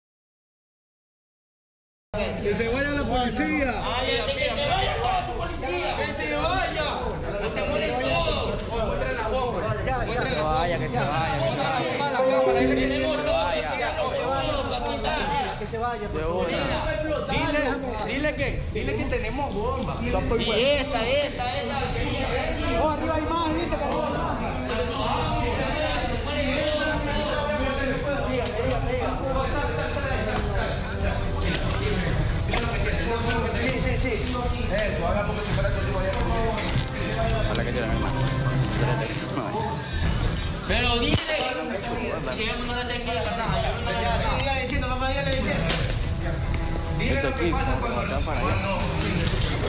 Moment masked gumen threaten TV presenter live on air with guns & dynamite
Live television images on Tuesday (January 9) showed hooded people inside Ecuador's TC television station in Guayaquil, pointing guns at the head of a TV anchor.

The take-over of TC's studio in Guayaquil was broadcast live for about 20 minutes, as people wearing balaclavas and mostly dressed in black wielded guns and accosted staffers huddling on the floor.

The hooded men pointed long and short weapons on several occasions at the Ecuadorean journalist Jose Luis Calderon, who was begging on camera for the police to leave the television station premises.

Police eventually rescued the TV staff and arrested the 13 men who burst into TC's studio.